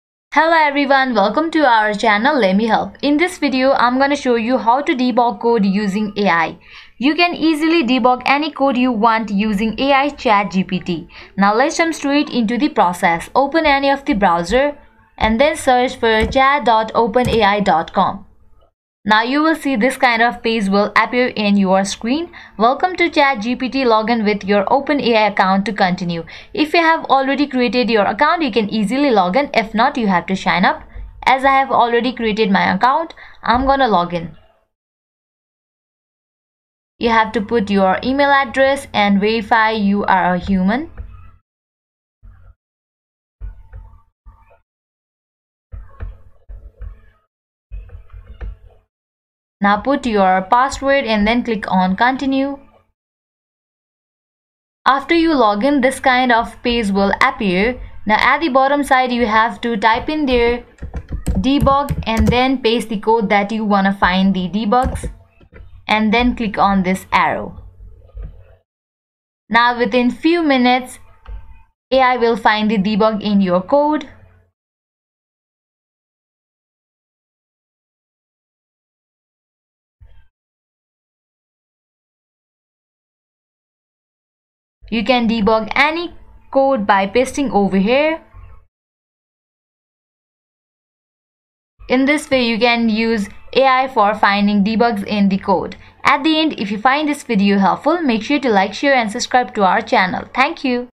How to Debug Code using AI
Artificial intelligence is intelligence—perceiving, synthesizing, and inferring information—demonstrated by machines, as opposed to intelligence displayed by animals and humans.
In this video we will learn how to debug code using artificial intelligence (AI). We can debug code find error and can get the right code for coding in AI. Follow us and learn to do it.